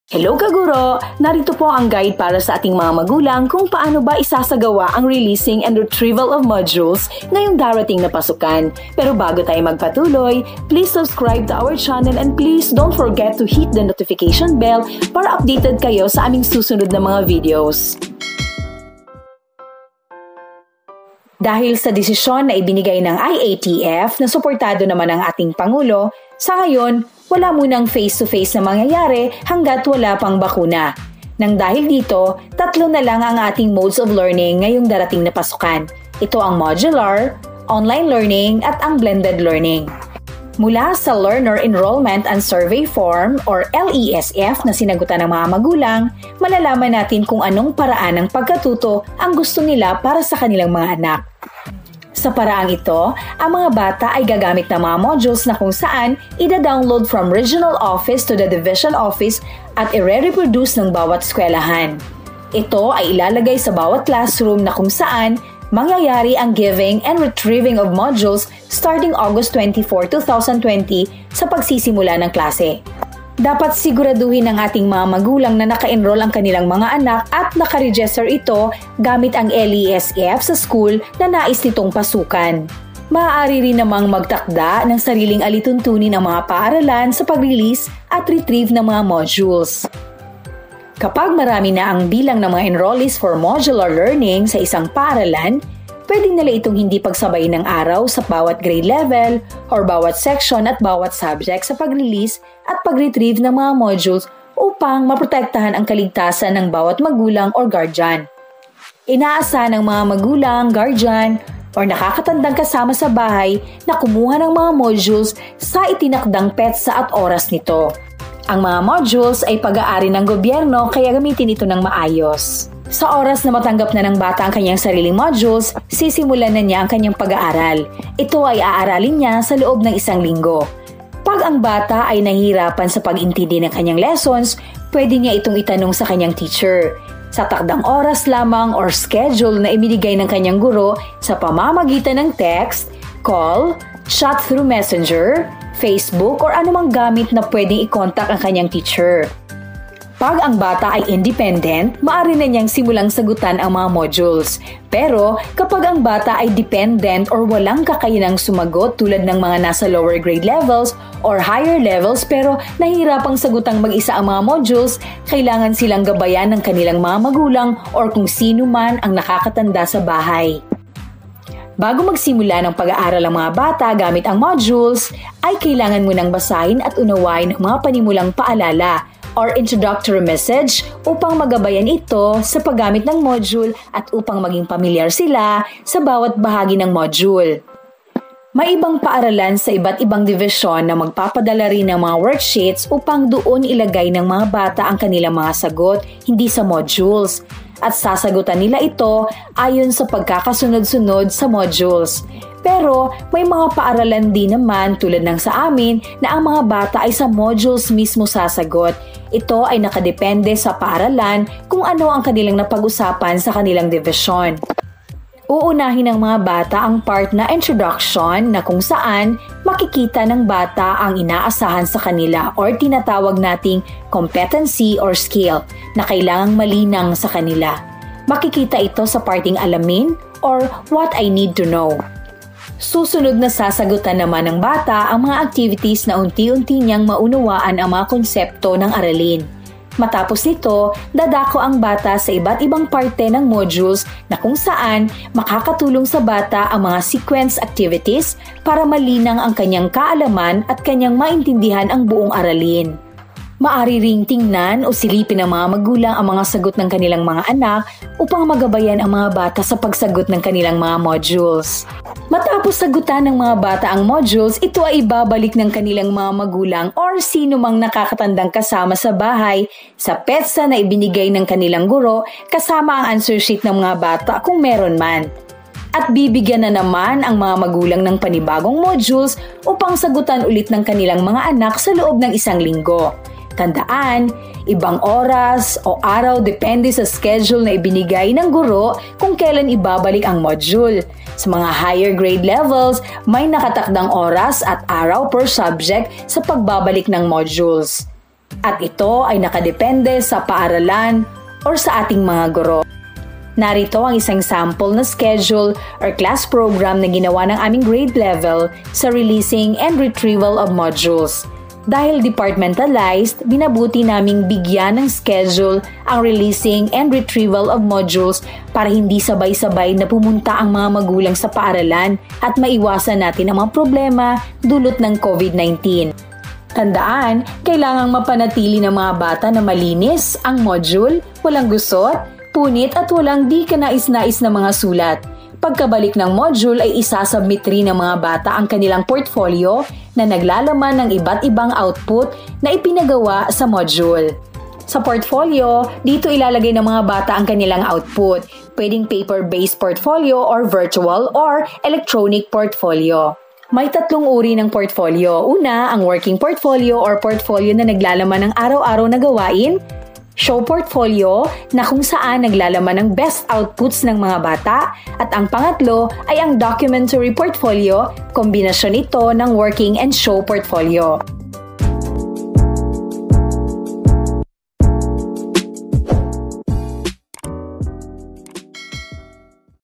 Releasing and Retrieval of MODULES on August 24, How? (Parents' Guide)
Paano isasagawa ang releasing at retrieval ng modules ngayong August 24?